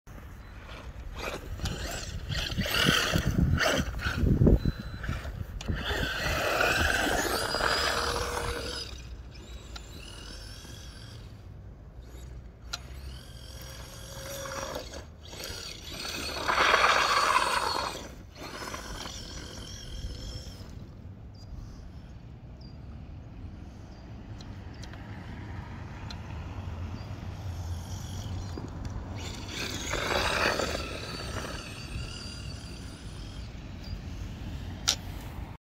Kleine Ausfahrt im Schnee Olympiastadion
Wenn es denn in Berlin schon mal Schnee gibt muss man den auch ausnutzen,
Eigentlich wollte ich ja auf den Parkplatz vor dem Olympiastadion, aber da waren welche mit ihren AMG und M3 1:1 Modellen am spielen.:-)